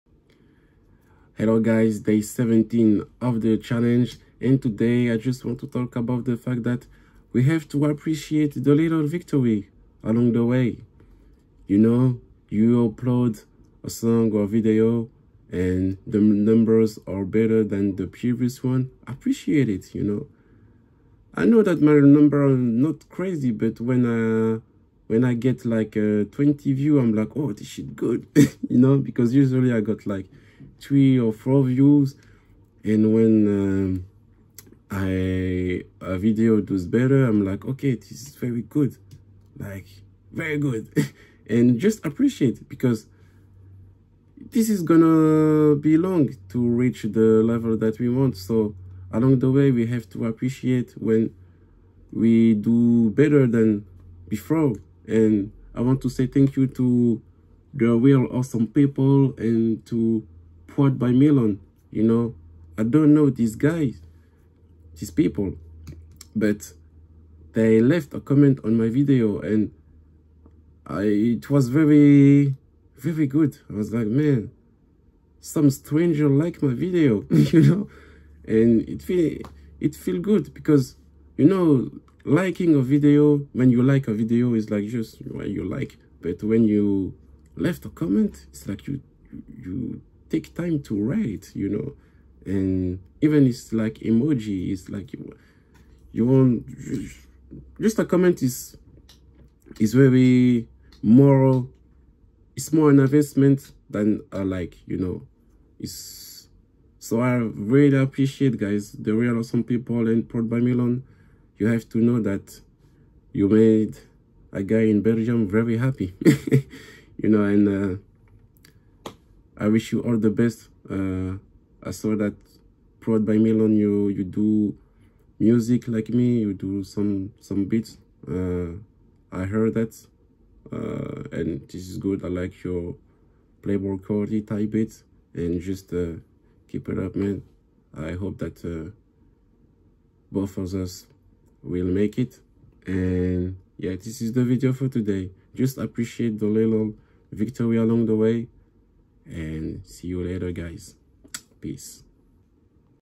The Crucial Role of Gratitude for Artists (day 17 of 30 talking to a camera challenge)
The Crucial Role of Gratitude for Artists !

let's appreciate the small victories as artist. This is something that we forget to do but it's very important in our journey.

Gratitude and appreciation is the key !